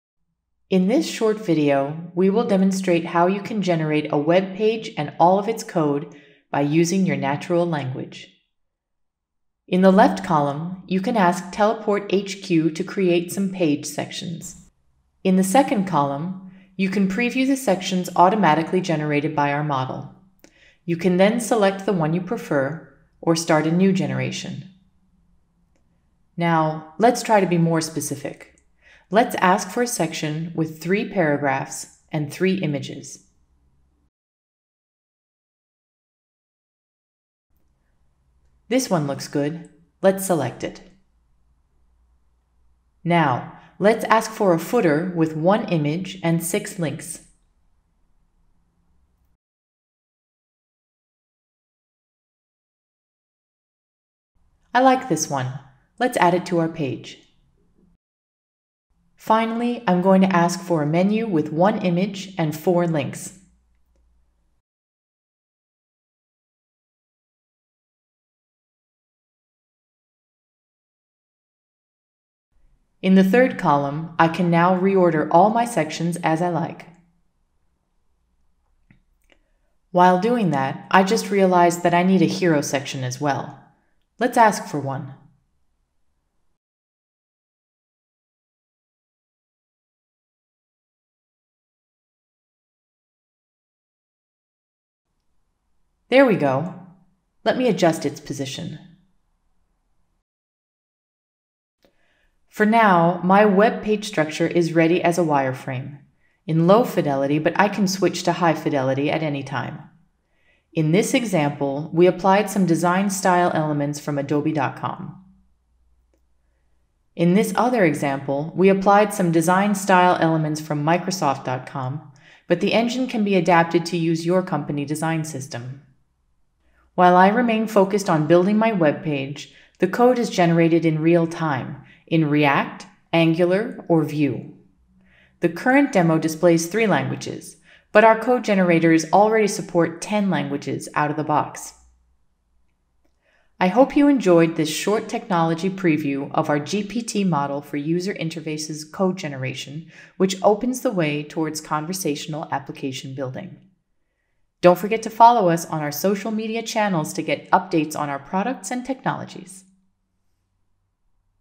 teleportHQ's GPT Engine for user-interfaces code generation
You can finally talk to your computer and get the source code of your user interface in real-time!